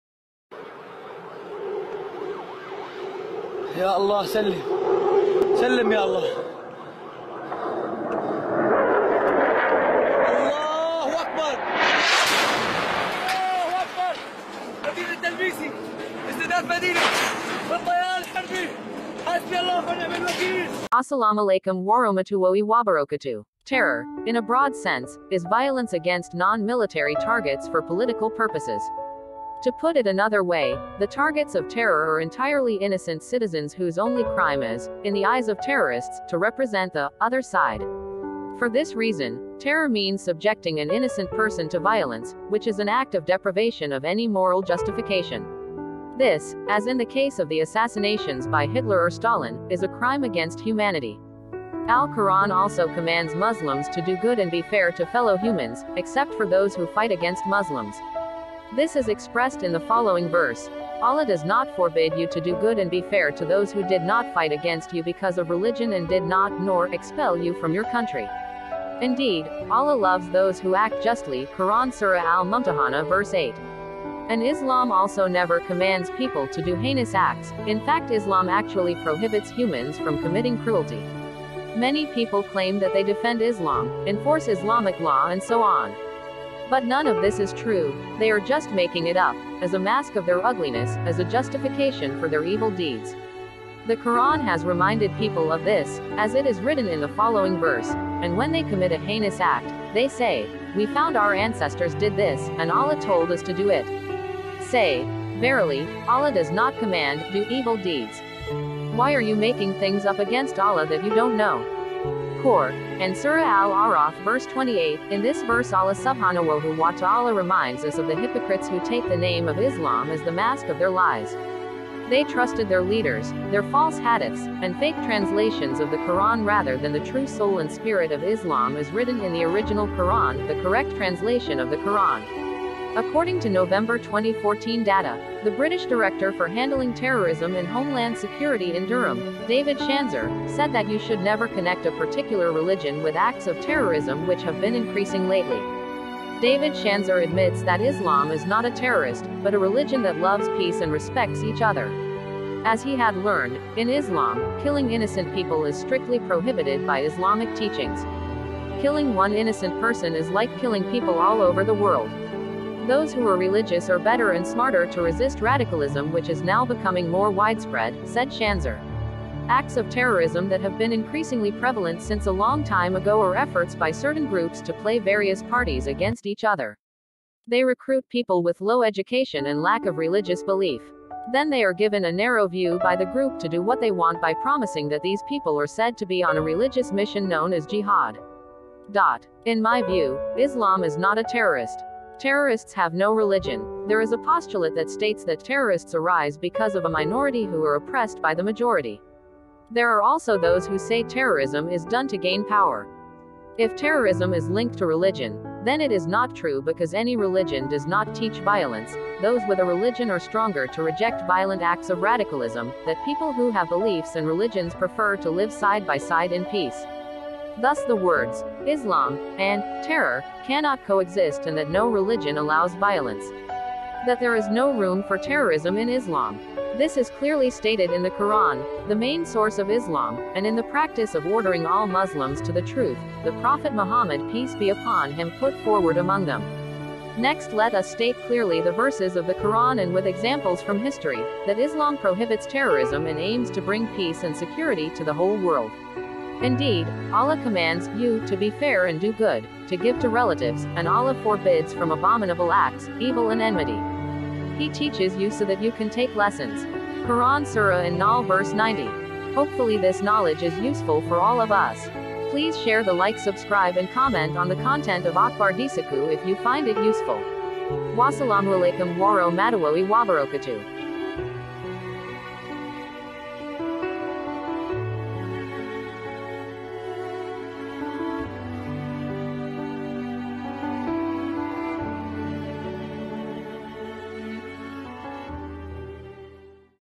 Islam Love Peace
Islam Love peace.In Islamic teachings, it always teaches the beauty of religious communities that mutual tolerance and love for peace is not war or terrorism.